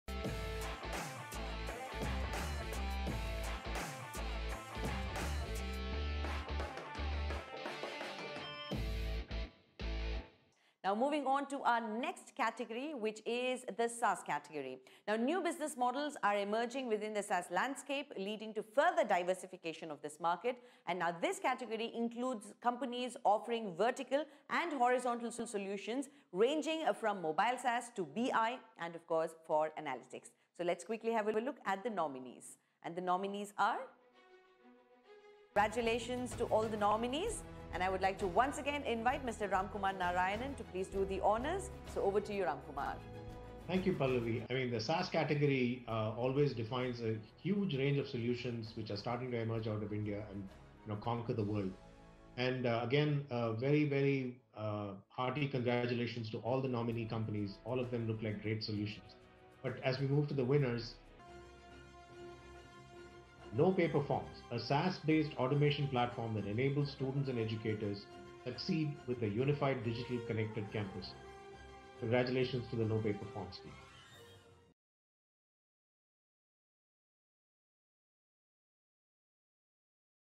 NoPaperForms crowned as one of the leaders in SaaS Landscape in India by Nasscom’s Emerge 50
Every year, NASSCOM evaluates over 500 companies to select the creme de la creme of the Indian Software and Product Industry. Emerge 50 is one among India's most prestigious & prominent awards in the tech space. The awards are synonymous with high caliber and innovative software products companies and many of the past winners have gone on to become unicorns.